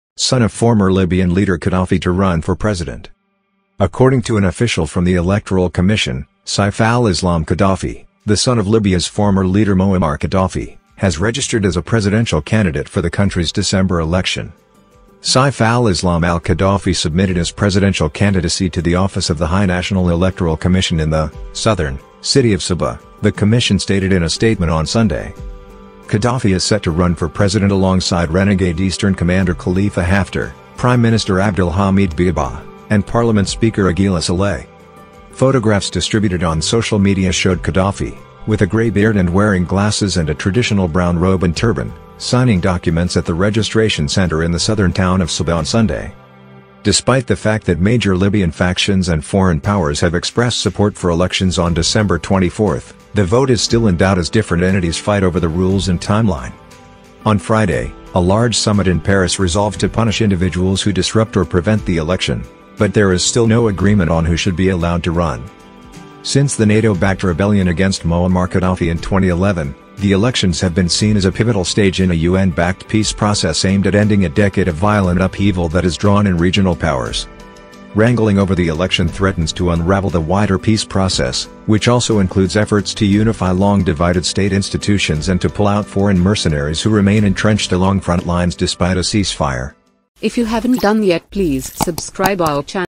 Son of former Libyan leader Gaddafi to run for president
According to an official from the electoral commission, Saif al-Islam Gaddafi, the son of Libya's former leader Muammar Gaddafi, has registered as a presidential candidate for the country's December election.

Saif al-Islam al-Gaddafi submitted his presidential candidacy to the office of the High National Electoral Commission in the [southern] city of Sebha, the commission stated in a statement on Sunday.

Gaddafi is set to run for president alongside renegade eastern commander Khalifa Haftar, Prime Minister Abdul Hamid Dbeibah, and parliament Speaker Aguila Saleh.

Photographs distributed on social media showed Gaddafi, with a grey beard and wearing glasses and a traditional brown robe and turban, signing documents at the registration centre in the southern town of Sebha on Sunday.

Despite the fact that major Libyan factions and foreign powers have expressed support for elections on December 24, the vote is still in doubt as different entities fight over the rules and timeline.

On Friday, a large summit in Paris resolved to punish individuals who disrupt or prevent the election, but there is still no agreement on who should be allowed to run.

Since the NATO-backed rebellion against Muammar Gaddafi in 2011, the elections have been seen as a pivotal stage in a UN-backed peace process aimed at ending a decade of violent upheaval that has drawn in regional powers.

Wrangling over the election threatens to unravel the wider peace process, which also includes efforts to unify long-divided state institutions and to pull out foreign mercenaries who remain entrenched along front lines despite a ceasefire.